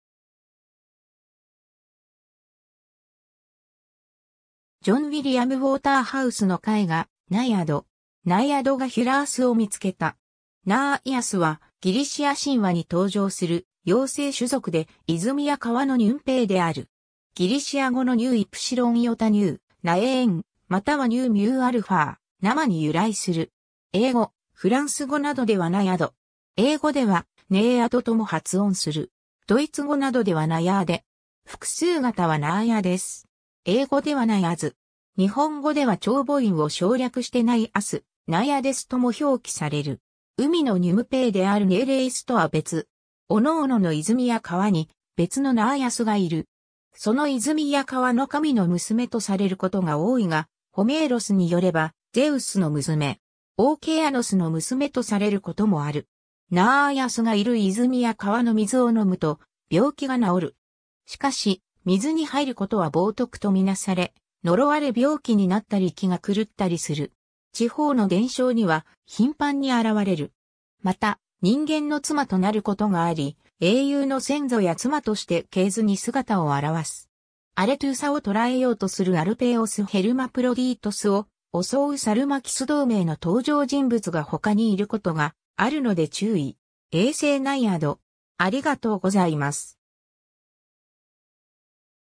ナーイアス
ジョン・ウィリアム・ウォーターハウスの絵画『ナイアド（ヒュラスとニンフ）』(1893年, 個人所蔵)。
ナイアドがヒュラースを見つけた。
 ナーイアス（古希: Ναιάς, Nāïas）は、ギリシア神話に登場する妖精種族で、泉や川のニュンペーである。
ギリシア語のνάειν, naein （流れ）またはνἃμα, nama （流水）に由来する。
英語、フランス語などではナイアド（英: Naiad、仏: Naïad）。
英語ではネイアドとも発音する。
ドイツ語などではナヤーデ（Najade）。
 複数形はナーイアデス（古希: Ναιάδες, Nāïades）。
英語ではナイアズ（Naiades）。
 日本語では長母音を省略してナイアス、ナイアデスとも表記される。
海のニュムペーであるネーレーイス（ネレイド）とは別。
 各々の泉や川に別のナーイアス（1人または複数）がいる。
その泉や川の神の娘とされることが多いが、ホメーロスによればゼウスの娘。
オーケアノスの娘とされることもある。
 ナーイアスがいる泉や川の水を飲むと、病気が治る。
しかし、水に入ることは冒涜とみなされ、呪われ病気になったり気が狂ったりする。
 地方の伝承には頻繁に現れる。
また、人間の妻となることがあり、英雄の先祖や妻として系図に姿を現す。
 アレトゥーサを捕らえようとするアルペイオス ヘルマプロディートスを襲うサルマキス 同名の登場人物が他にいることがあるので注意。
 衛星ナイアド